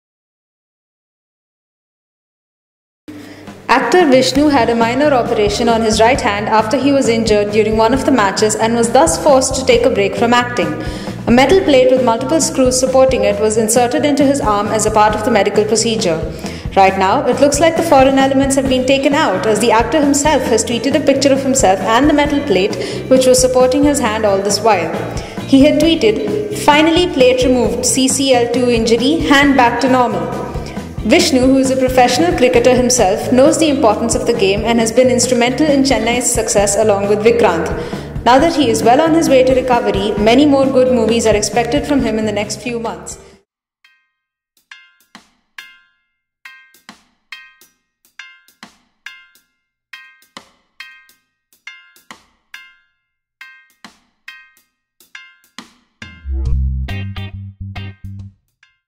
Actor Vishnu Recovers His Operation | Silly Monks
This proves that celebrities took CCL seriously that has greatly explore their career. The cricket tournament that is meant solely for film stars from across the nation was began on the lighter note but have collected prominence and pace through the years.The star player, literally the ‘star’ player of Chennai Rhinos, actor Vishnu is presently going through treatment inside a private hospital because of an injuries he acquired while playing cricket within the Celebrity Cricket League(CCL).Since the Jeeva actor is well on his method to recovery, a lot more good movies is anticipated from him within the next couple of several weeks. Stay updated for more updates around the actor’s approaching movies.